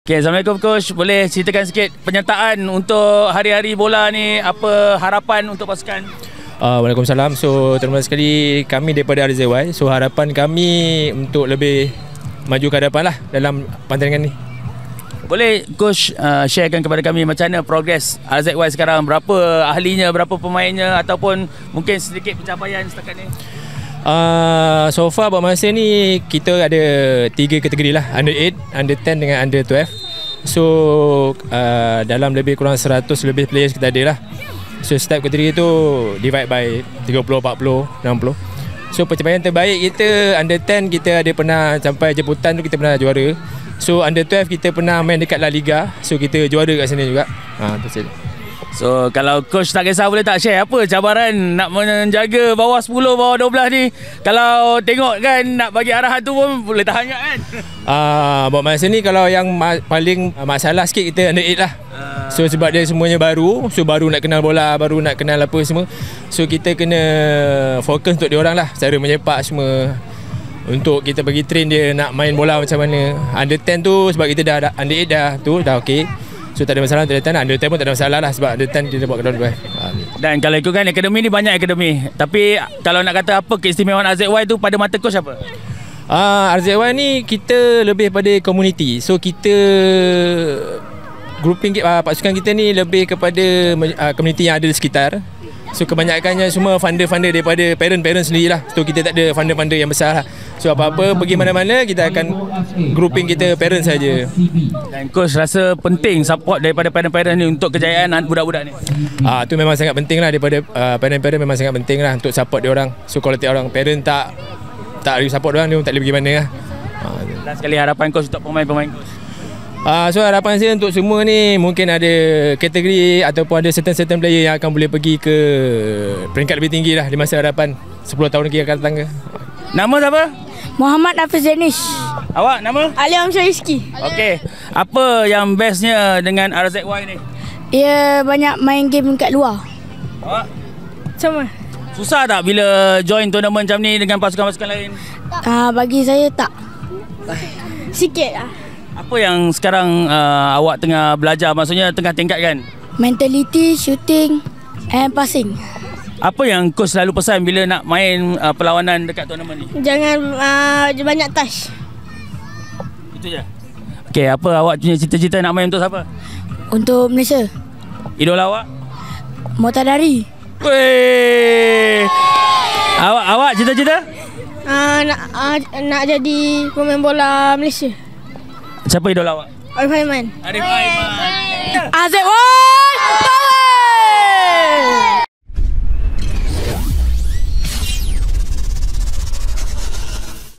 "Parents yang banyak jadi 'funder'! Tanpa sokongan dari parents, pemain pemain takkan ke mana!"
Temubual bersama jurulatih, Mohd Azam Bin Razali dan pemain pemain RZY Football Club mengenai perkembangan pasukan dan harapan untuk masa hadapan.